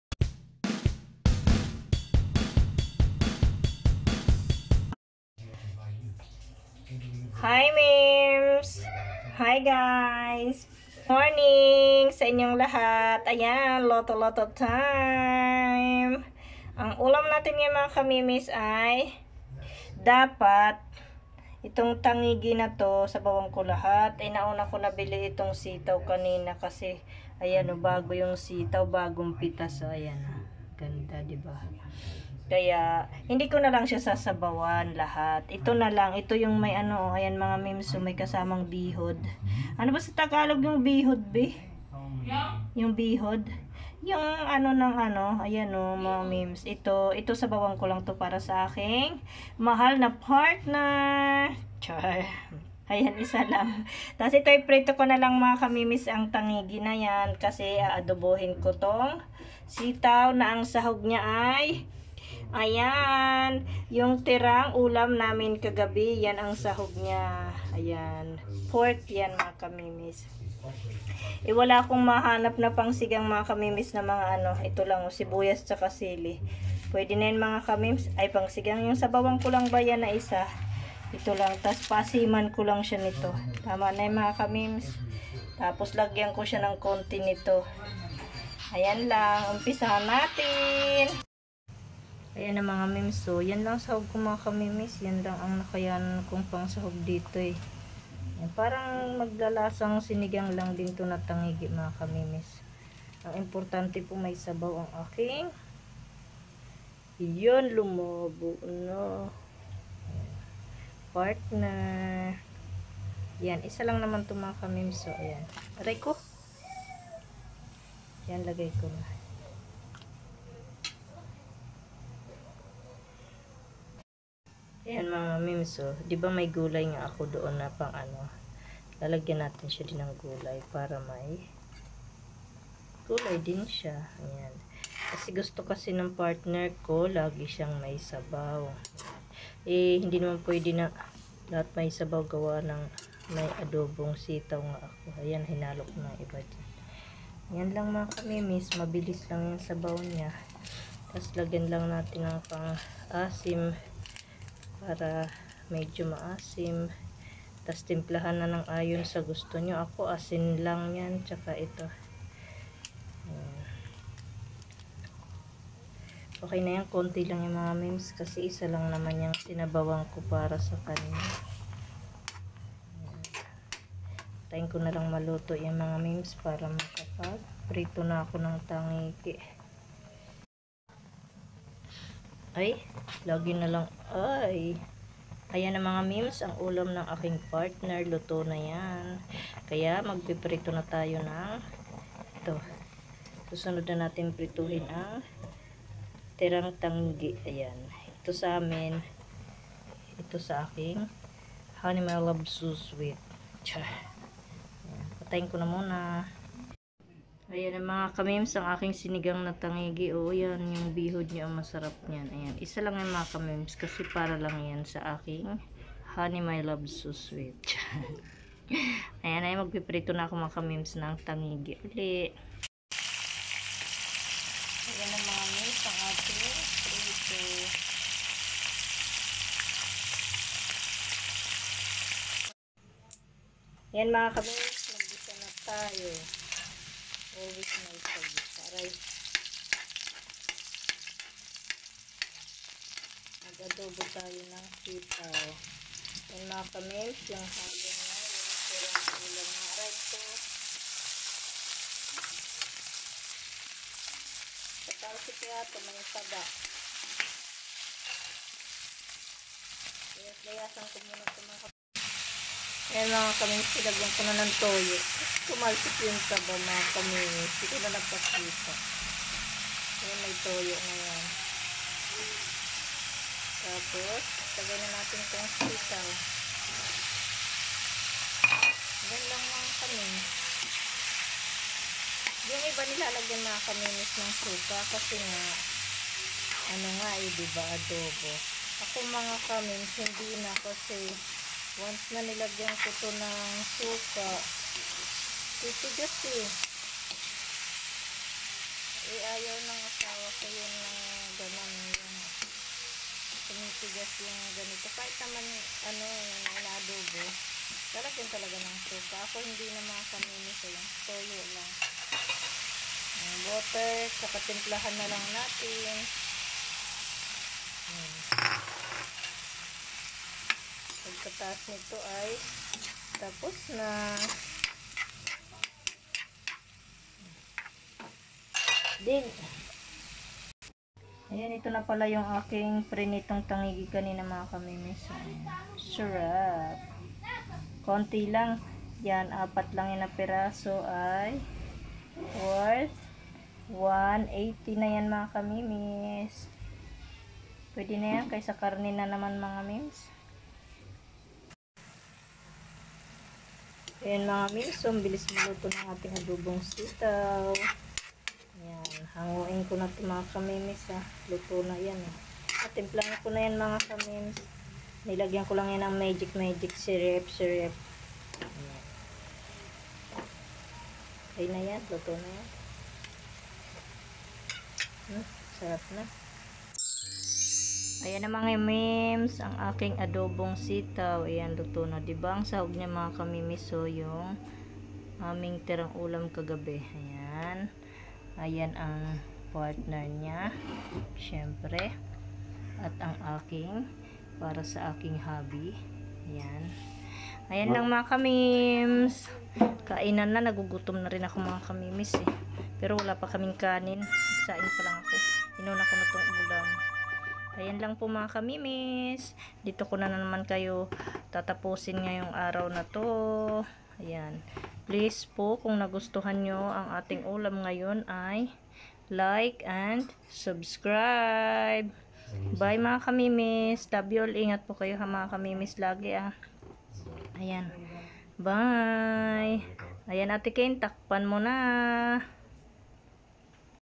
ADOBONG SITAW (+ Fresh TANIGUE)kain na mga kamemes
Mga kamemes hello po sa inyong lhat kamusta po kayo sana ay nsa mabuting kalusugan po tayong lhat,

Luto TYO ng adobong sitaw at pritong tangegue masarap at masustsnsya, try nyo na  po,,

WANT TO VISIT MY FACEBOOK :GELY RAZA

BYE ❤️❤️❤️❤️😘🌹🌹🌹